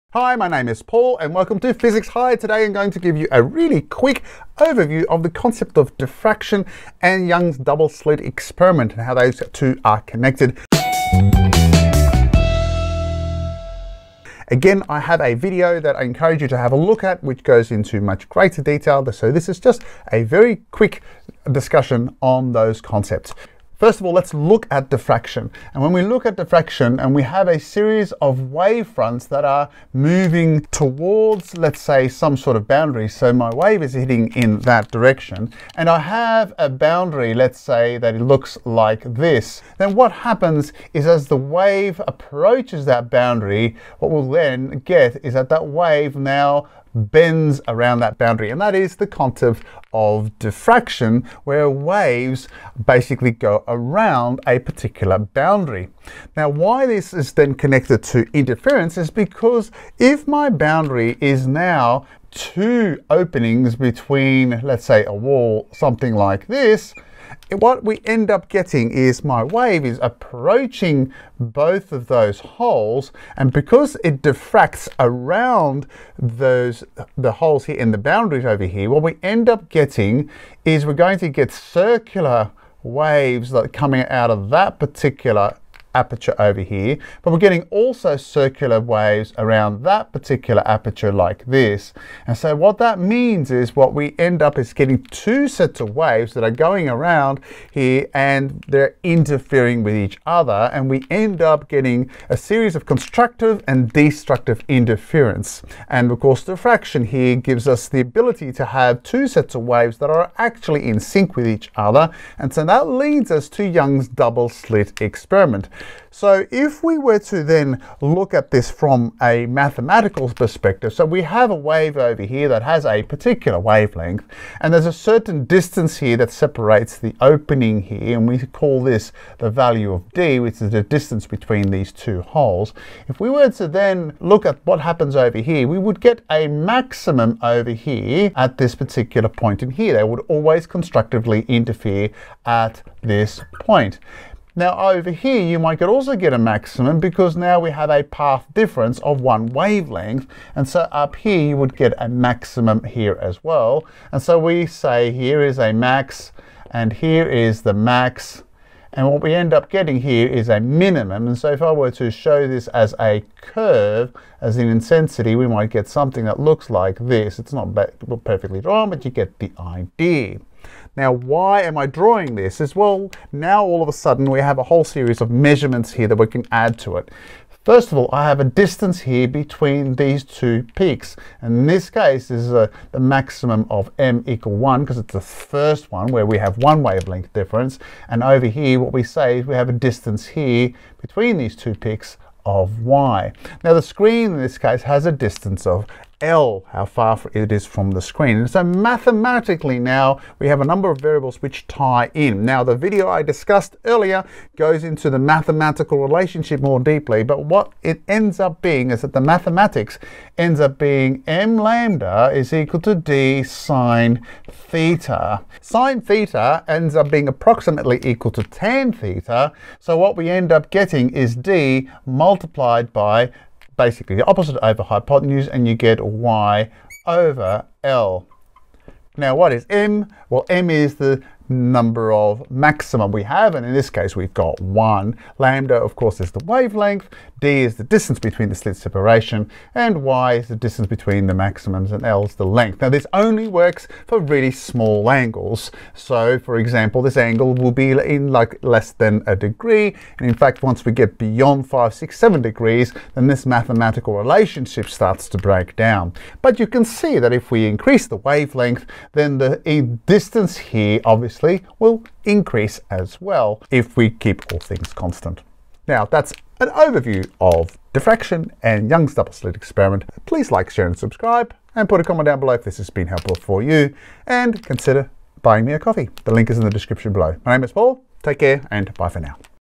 Diffraction and double slit interference in 4 minutes
In this brief video I summarise diffraction and interference that underpinns Young's double slit experiment.
The experiment led to evidence that light is a wave phenomenon.

See the following videos for further elaboration...